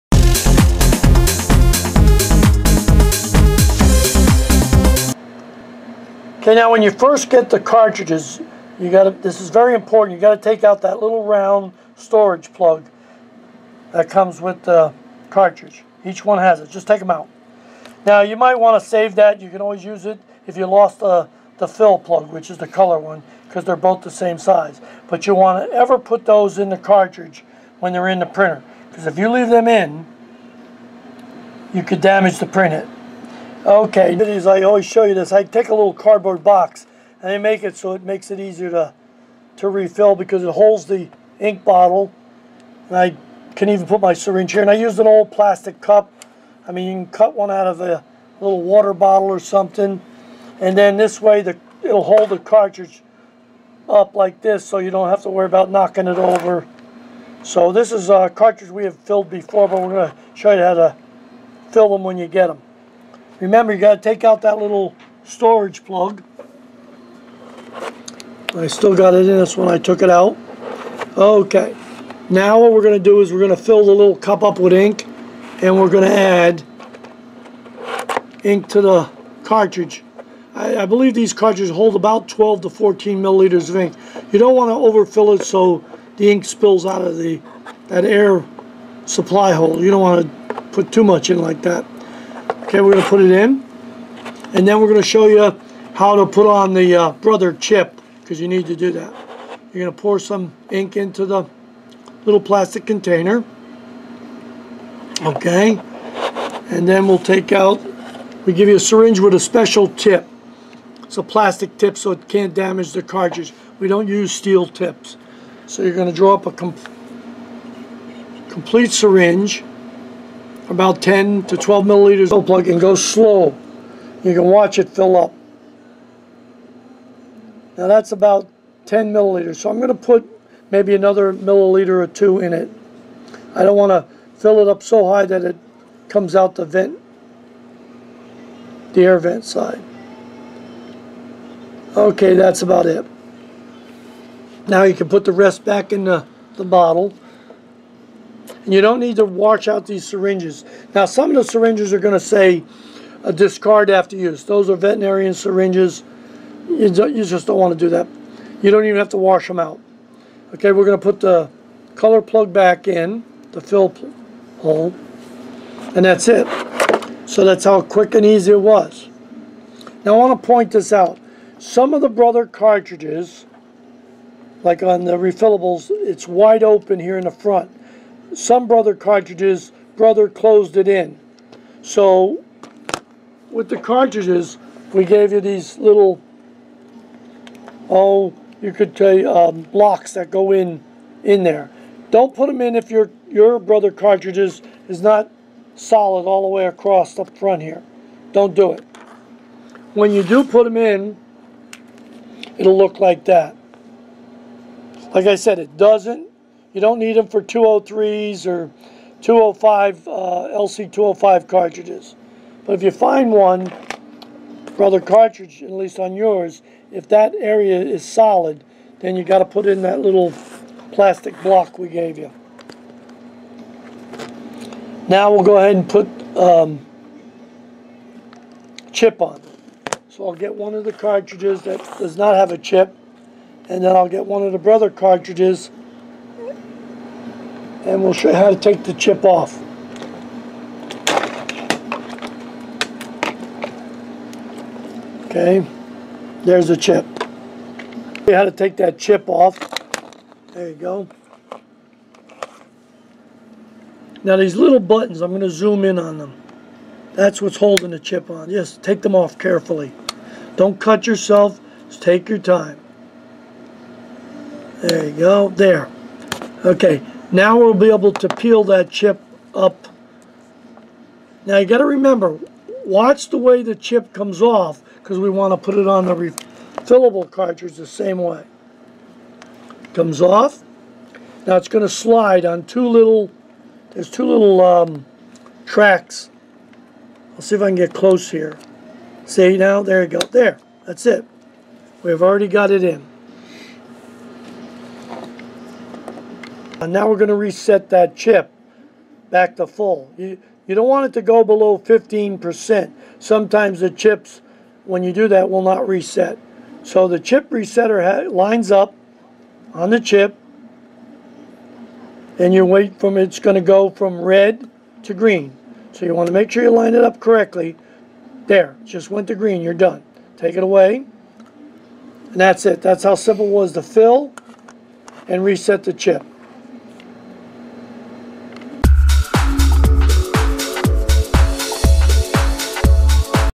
Brother Refillable LC-203-LC-205-LC-207 Cartridges With Refill Kit
3 - 60 ml bottles of Ultra True color ink™  Yellow,  Magenta,  Cyan
1 - 120 MLS of Ultra True Color Pigment Black ink ( waterproof ink )
1 - Set of Clear Super Size Refillable Cartridges
1 - COC Smart Chip™ Re-setter
4 - Syringes with safety tips for filling the cartridges.